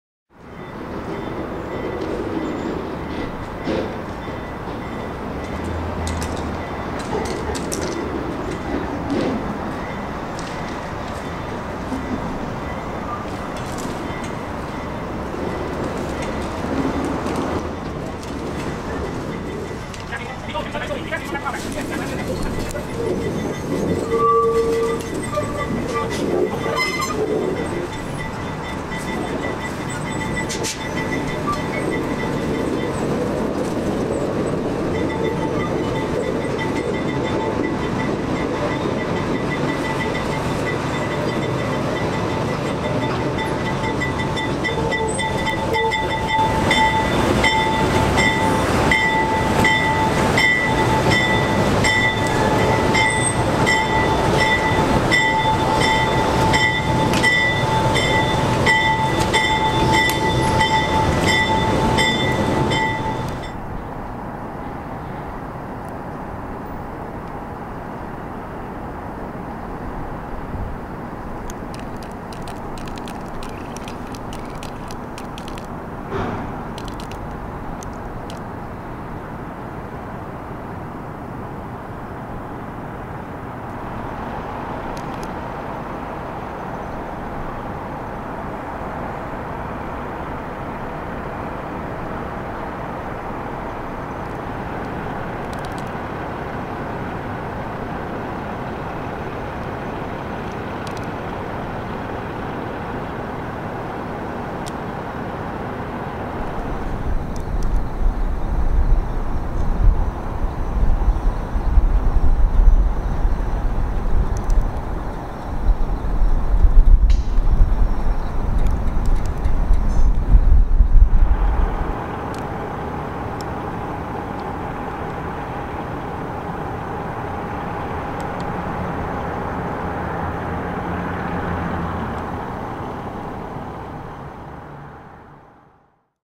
R110A Cab/A-Cars Awaiting Truck Shipment in Sunset Park for Pump Train Conversion - Jan. 4, 2022
Here are several videos taken on January 4, 2021, featuring the MTA NYC Subway - in particular, the South Brooklyn Railway. After a slight delay from mid-December 2021, four Kawasaki Heavy Industries R110A cars were towed tracks on the South Brooklyn Railway east of 3rd Ave to a transloading site west of 2nd Ave. The cars seen here are the four A-cars, which had cab controls on one end. While the six R110A B-cars were converted to pump train cars in the 2010s, the four R110A A-cars sat unused and mothballed until a decision was made to convert them to pump train cars as well. Unlike the B-cars, the A-cars were converted off-site at Brookville Equipment Corporation; as a result, they had to be loaded onto trucks to be taken off-site.